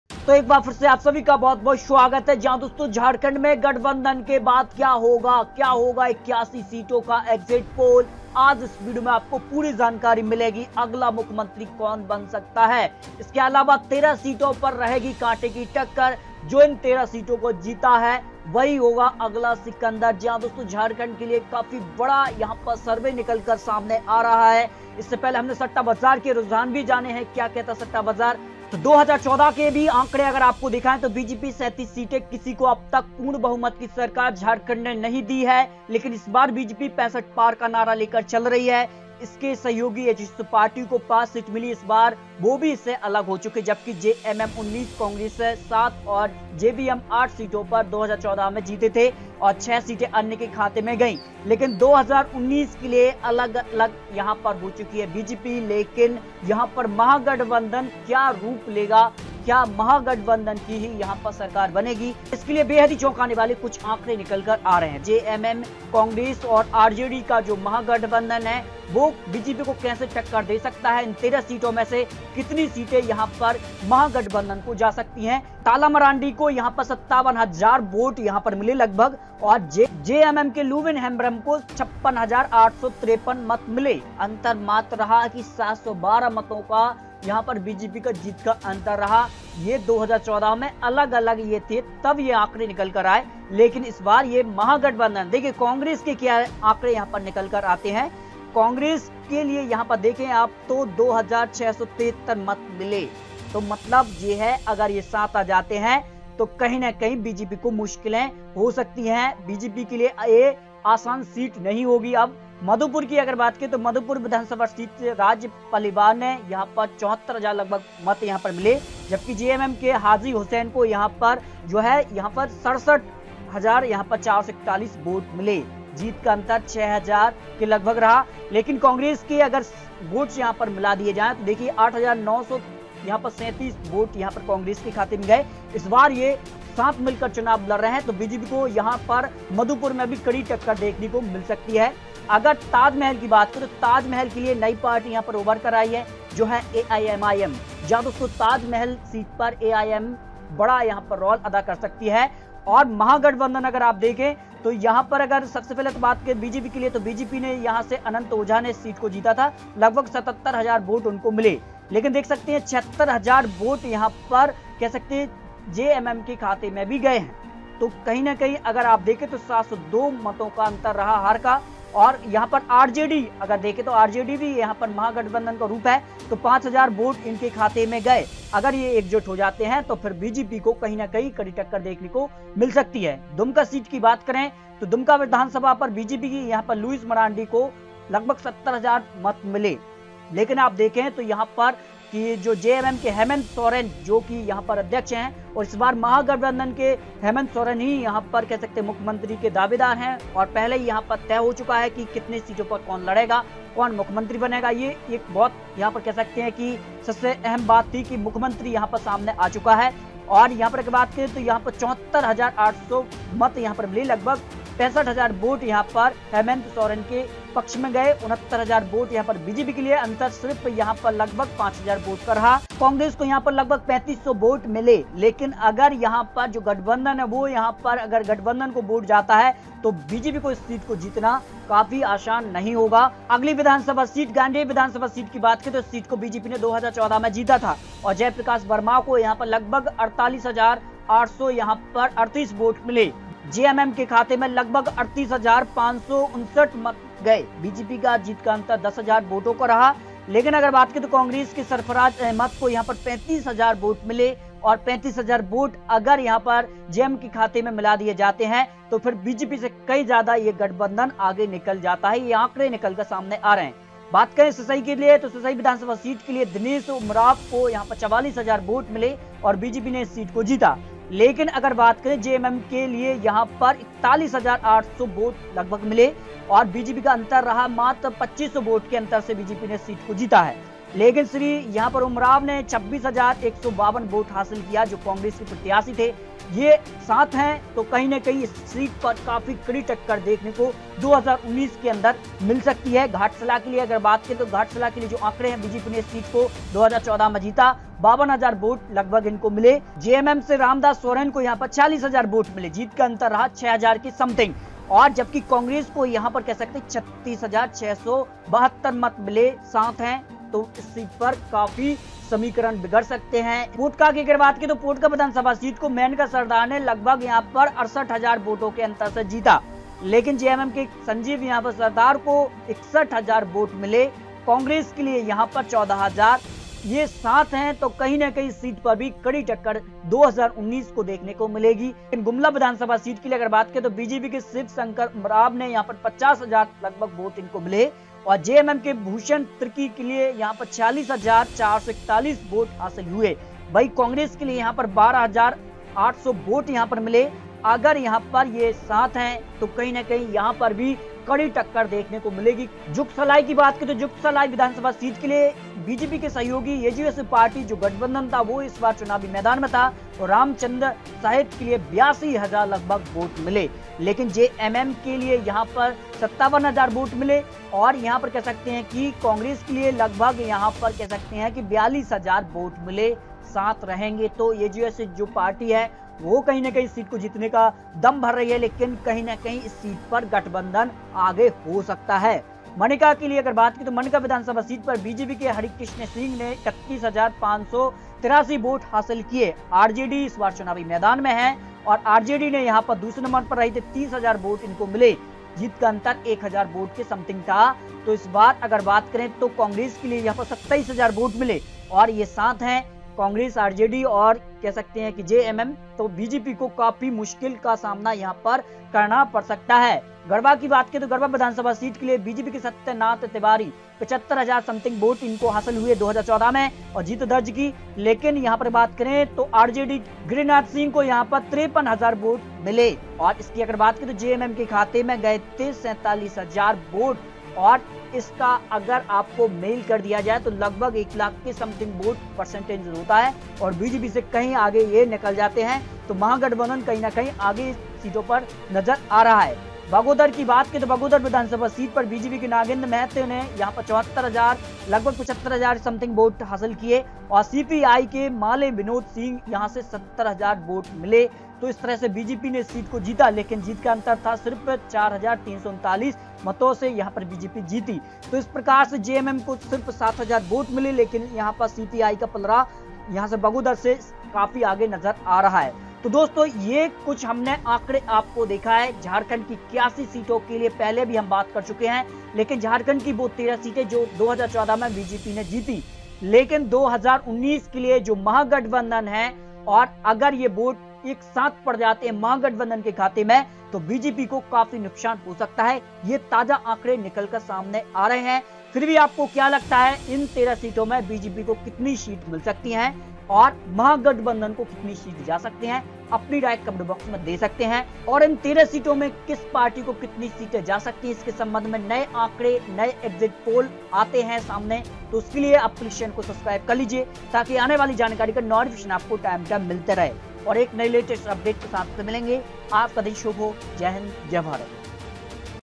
झारखंड गठबंधन के बाद |13 सीट जो जीता वही होगा अगला CM? कांटे की टक्कर 13 सीटों पर | ताजा Exit Poll
झारखंड गठबंधन के बाद |13 सीट जो जीता वही होगा अगला CM?
  कांटे की टक्कर 13 सीटों पर
      ताजा Exit Poll
 Breaking news today
kishan news
news 2019
new vacancy
bharti police
ipl news